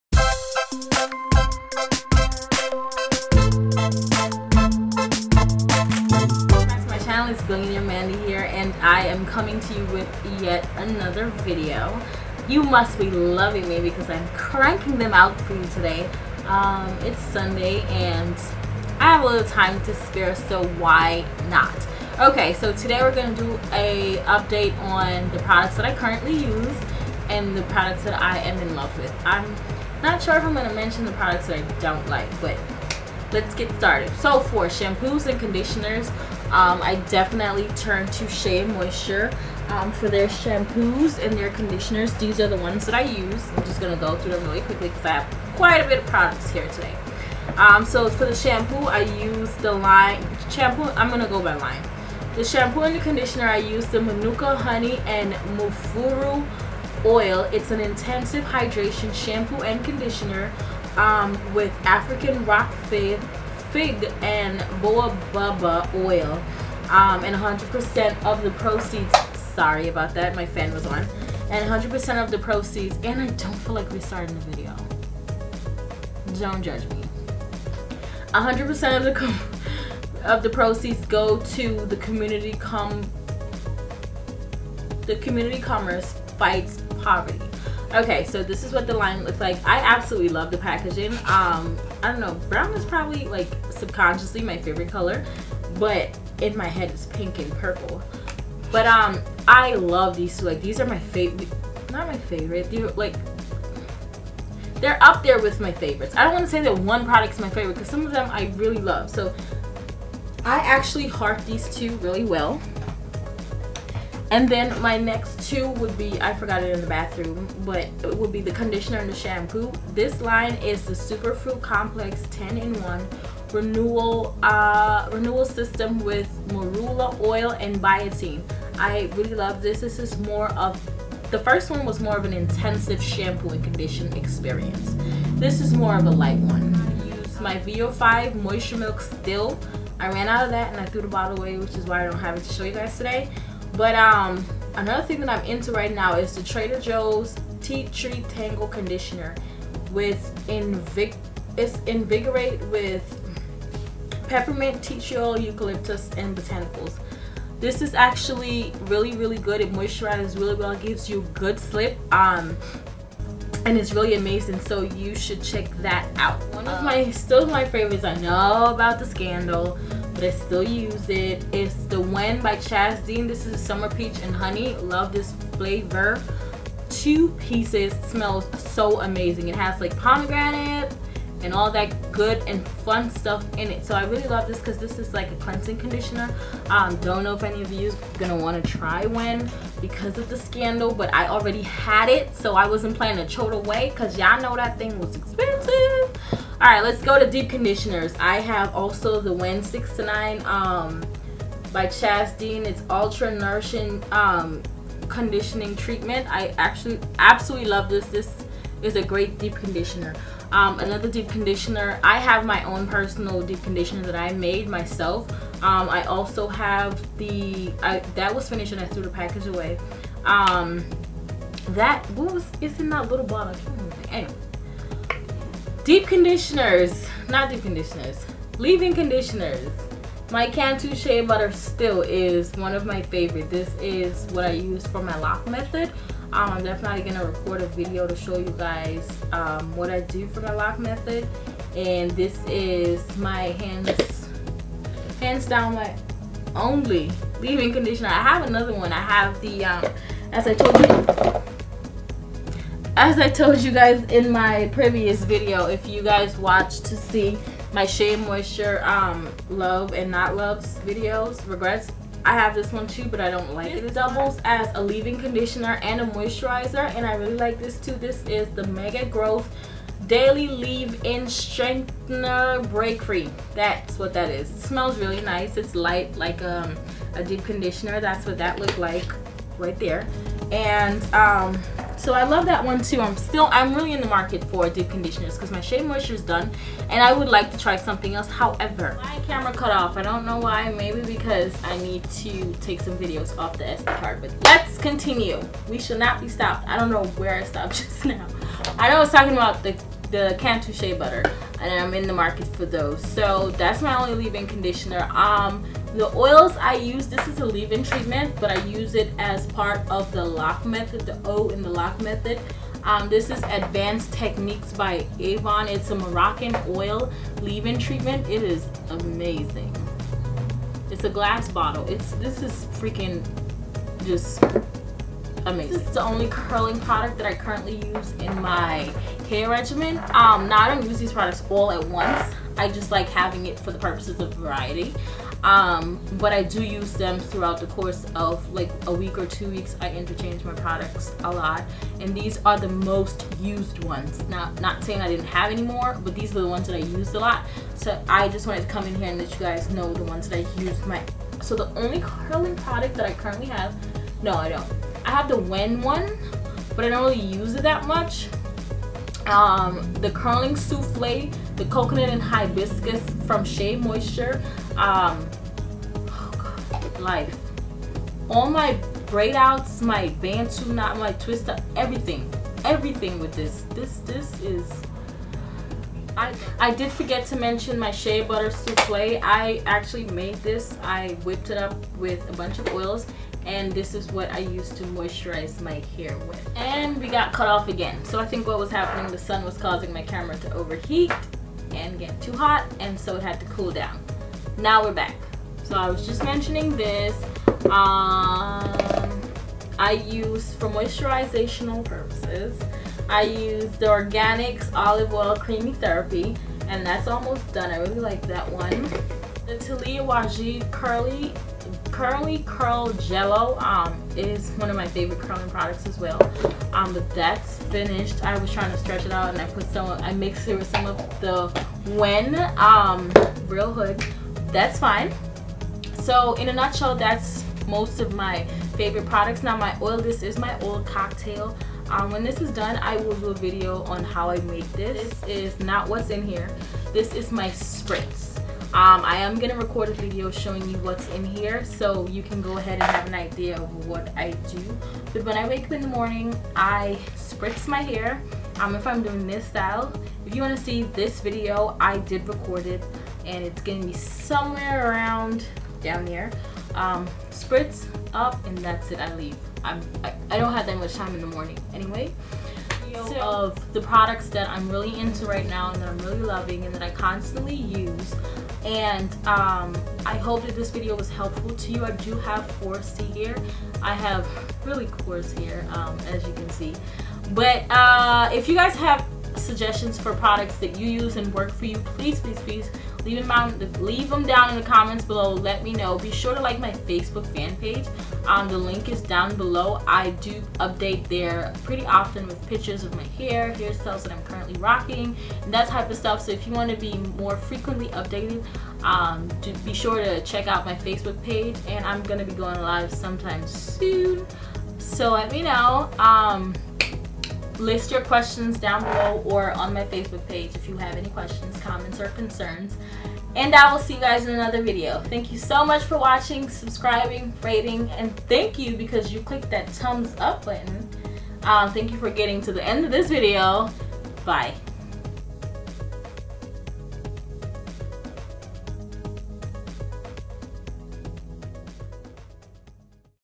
Hair Update 2016 :: Current Hair Faves::
Hey Loves, I know you are enjoying the frequent posts! I wanted to update you guys on what i currently use in my hair as part of my regimen. If you guys have any suggestions for great products that you stand by let me know and I will be sure to try them!

Protein Treatment : How to 101
htttp://youtu.be/KVAfavhgNrw

Are you on your hair Journey well check out where I am on my Journey